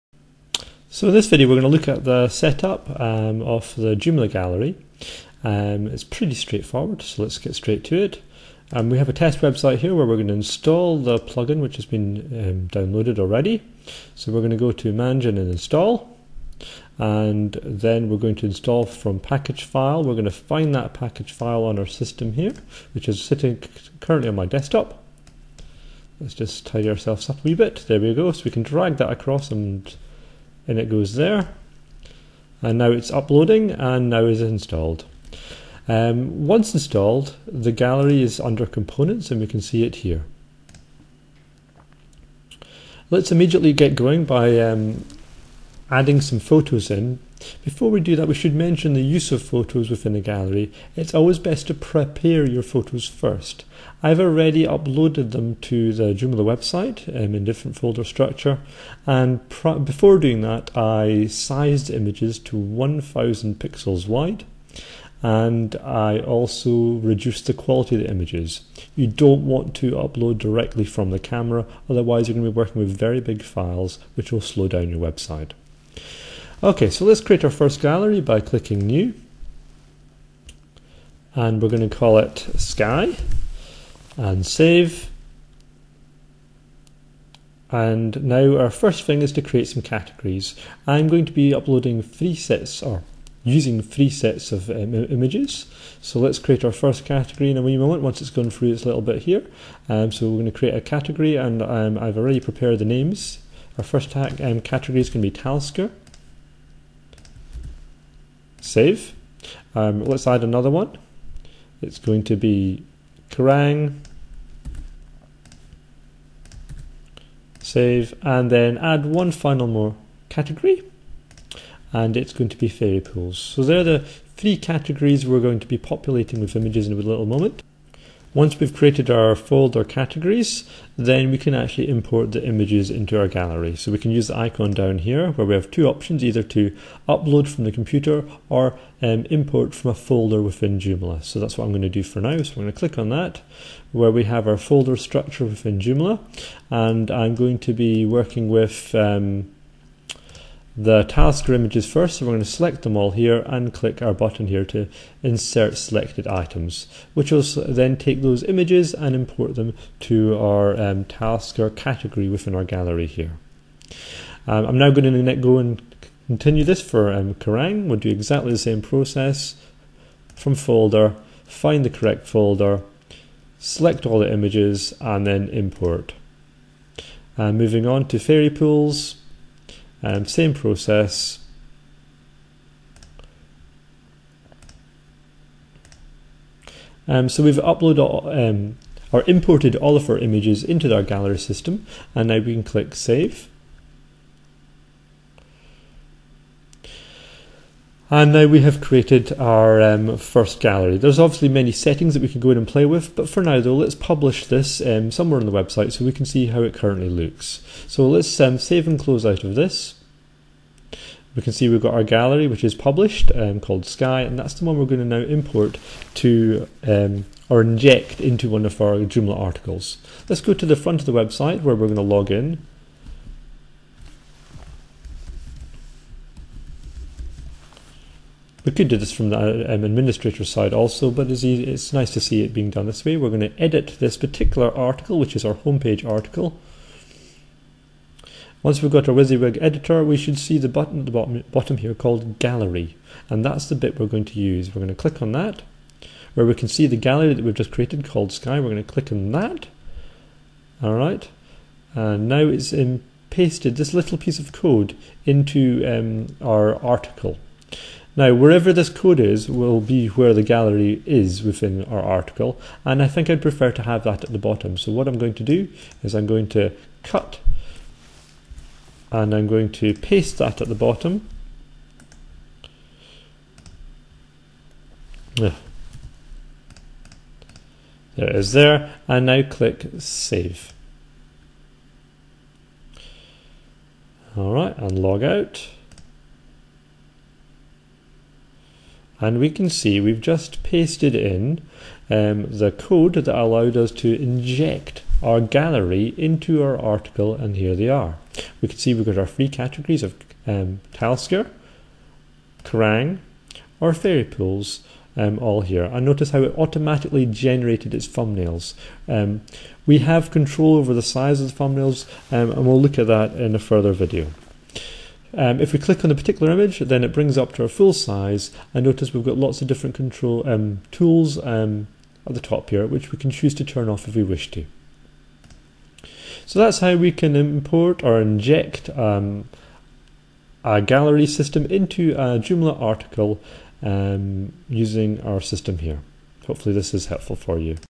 Balbooa Joomla Gallery | Install and Setup
How to install the Balbooa Joomla Gallery, add categories and import images then finally publish the gallery into a Joomla Article.